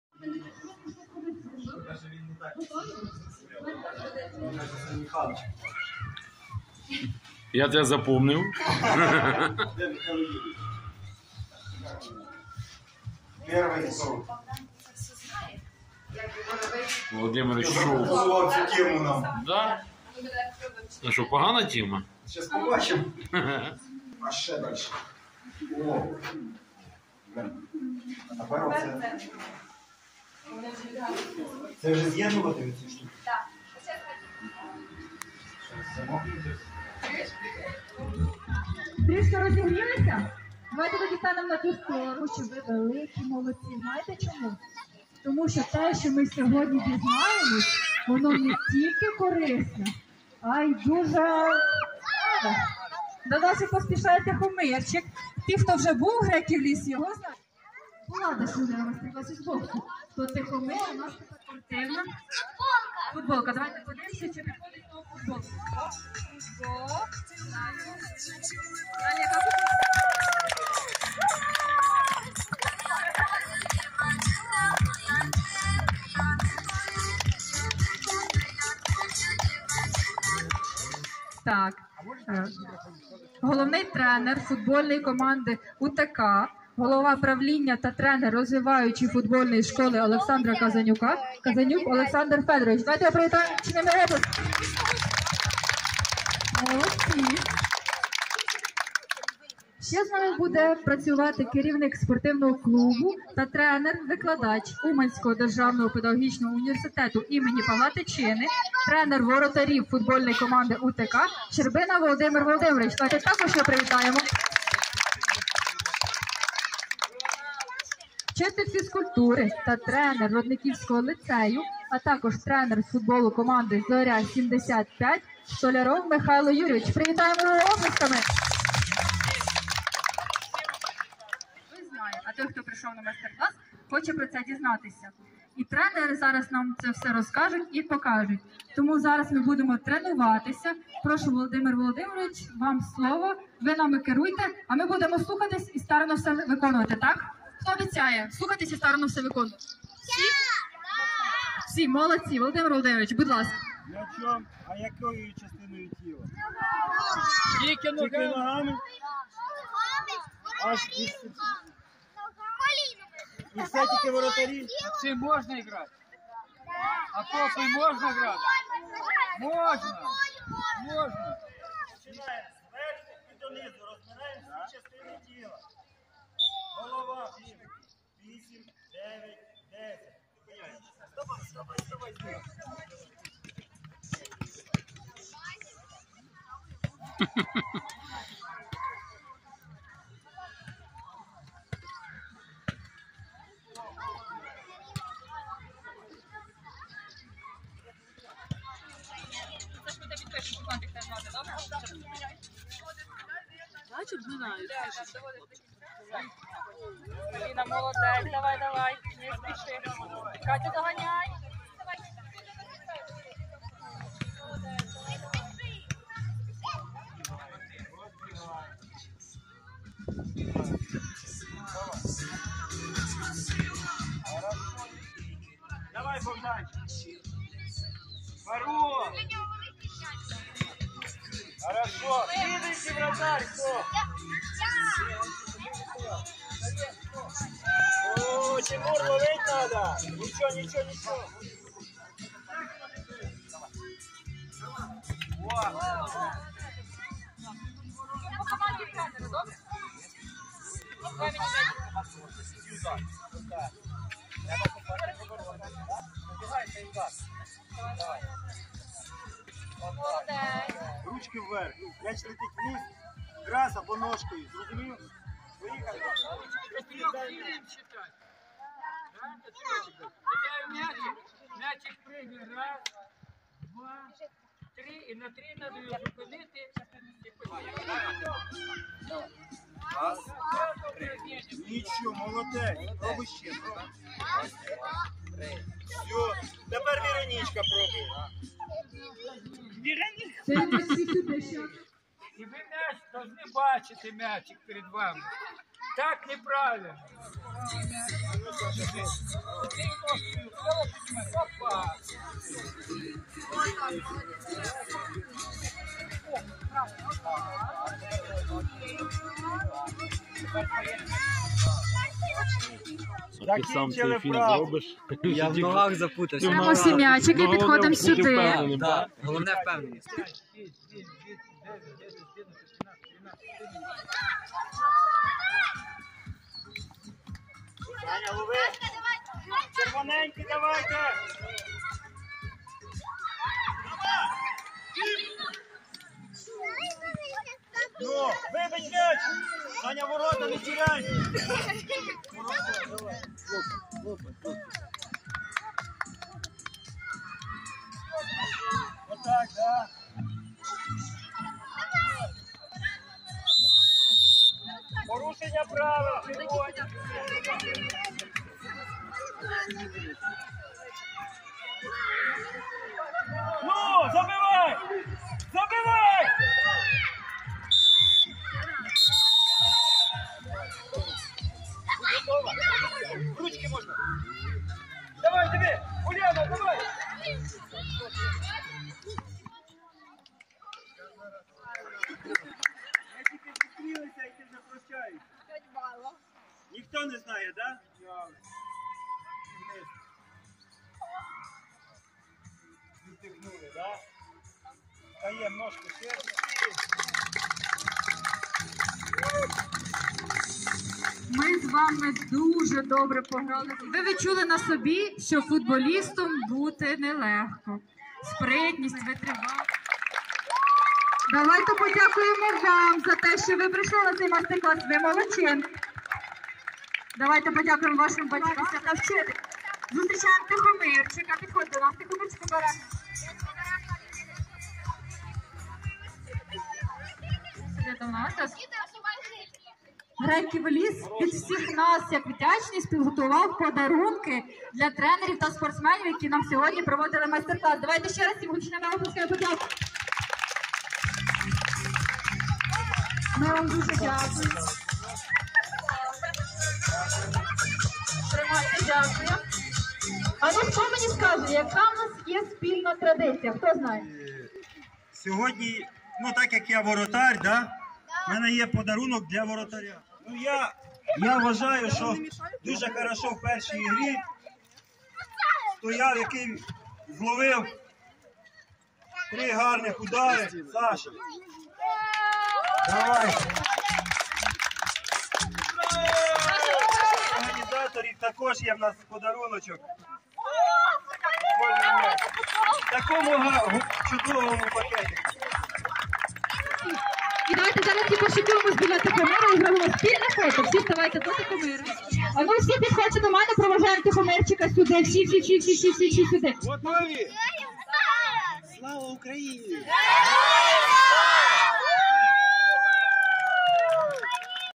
Майстер-клас для дітей. Греків ліс. 9 червня 2023
9 червня 2023 в ЖК Греків ліс відбувся майстер-клас для діток з футболу. Проводили його тренери ФК УТК, дитячої школи імені О.Казанюка і студенти УДПУ. Було весело, цікаво, пізнавально.